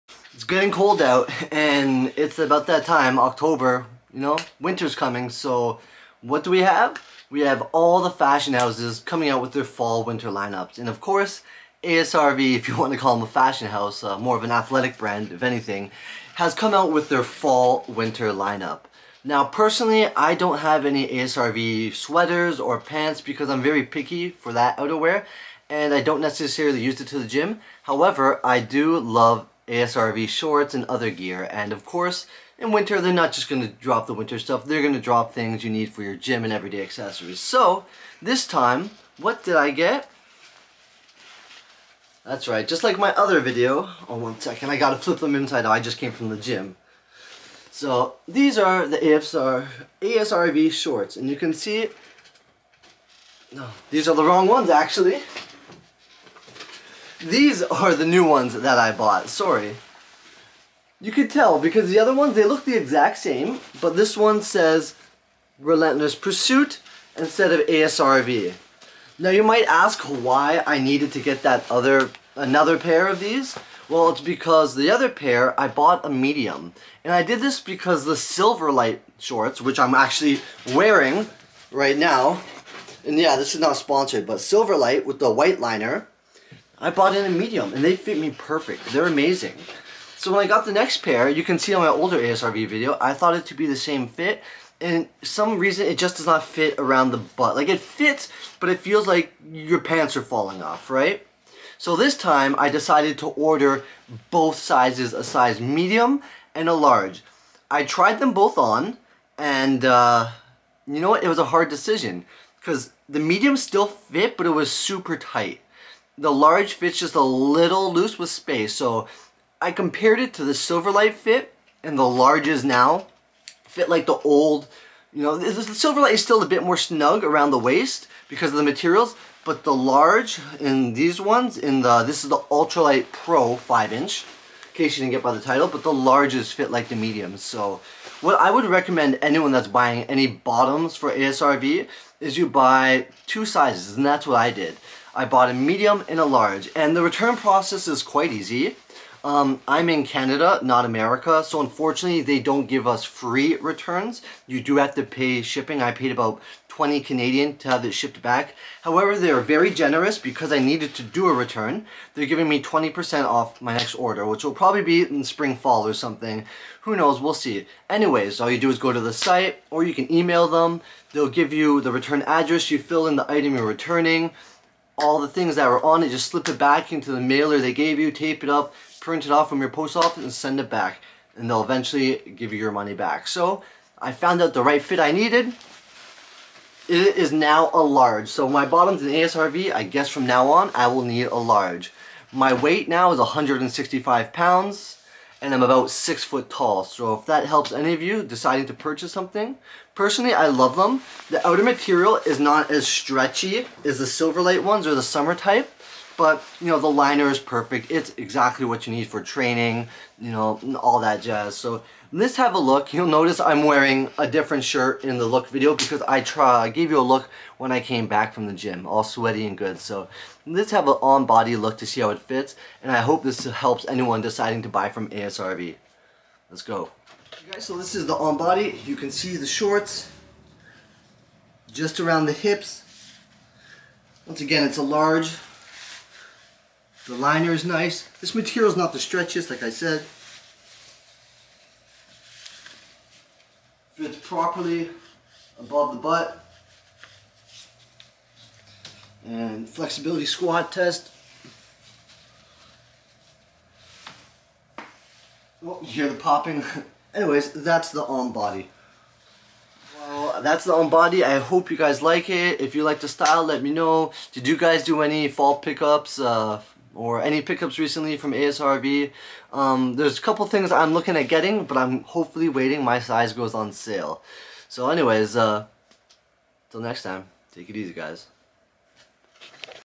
ASRV ultralight pro 5” liner short, size guide, review fall 2021
My pickup from the recent asrv fall drop 2021. I picked up a L in the ultralight pro 5 inch liner short. I am 6 foot 165 lbs.